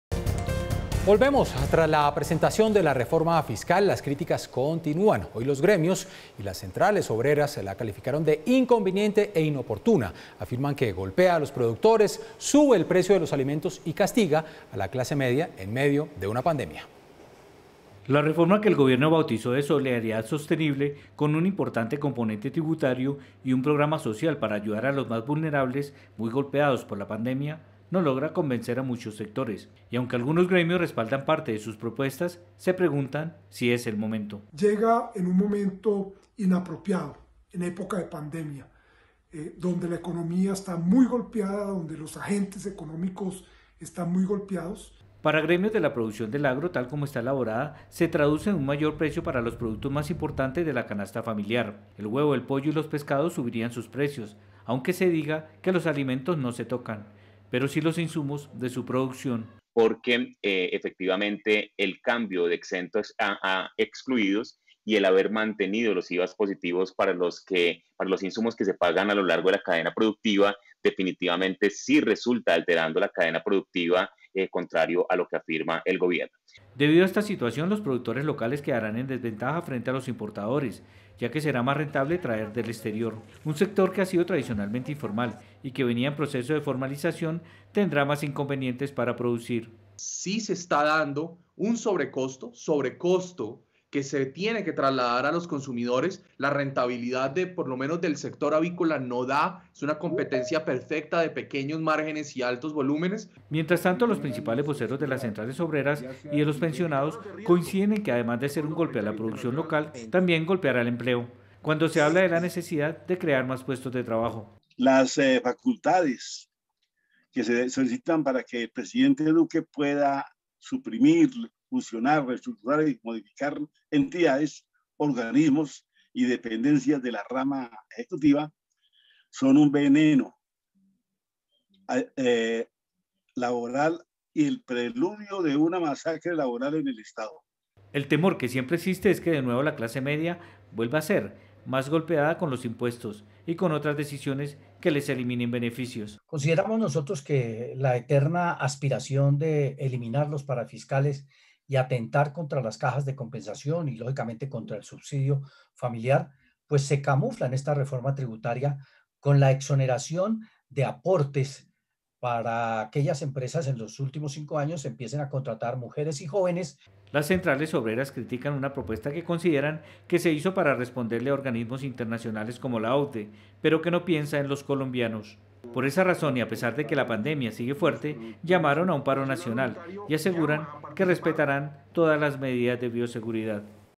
Red+ | La reforma fiscal aún no logra convencer
Tras la presentación de la reforma fiscal las críticas continúan. Hoy gremios y las centrales obreras la calificaron de inconveniente e inoportuna que golpea a los productores, sube el precio de los alimentos y castiga a la clase media en medio de una pandemia.